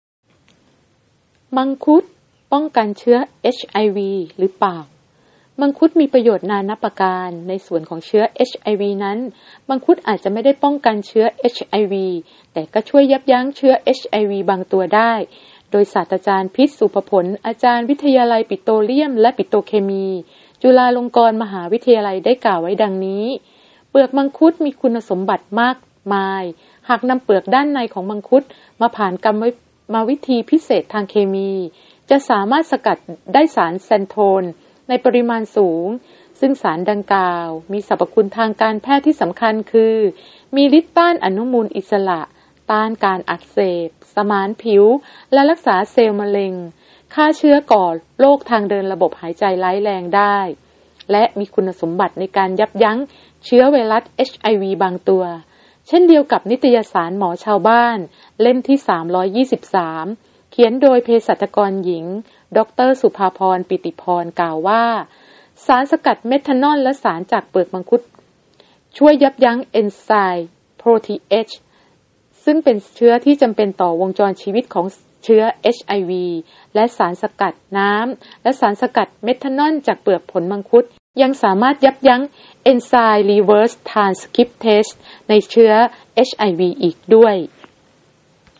มังคุดป้องกันเชื้อ HIV ได้จริงหรือไม่? [mcmHealth]
มังคุดมีประโยชน์นานัปการ ในส่วนของเชื้อเอชไอวี  นั้น มังคุดอาจจะไม่ได้ป้องกันเชื้อ HIV แต่ก็ช่วยยับยั้งเชื้อ HIV บางตัว โดย ศ.พิชญ์ ศุภผล อาจารย์วิทยาลัยปิโตรเลียมและปิโตรเคมี จุฬาลงกรณ์มหาวิทยาลัย ได้กล่าวไว้ดังนี้